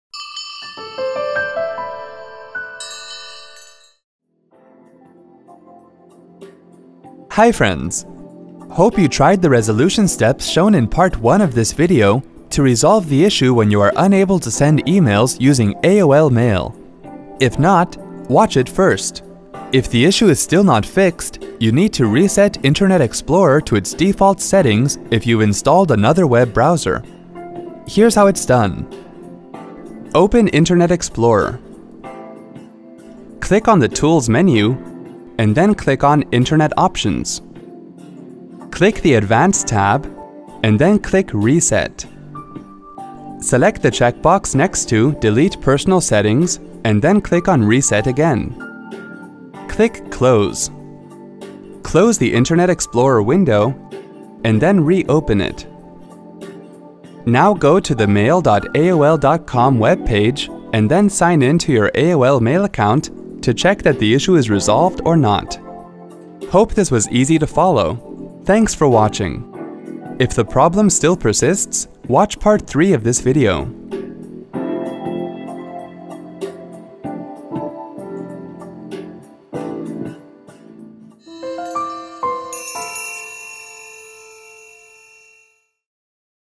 Unable to send e-mails using AOL® Mail (Part-2)
Are you facing problems in using your AOL® Mail account to send emails to your friends and family? If you want to resolve the issue when you are unable to send e-mails using AOL® Mail, watch this video and follow the steps to do so.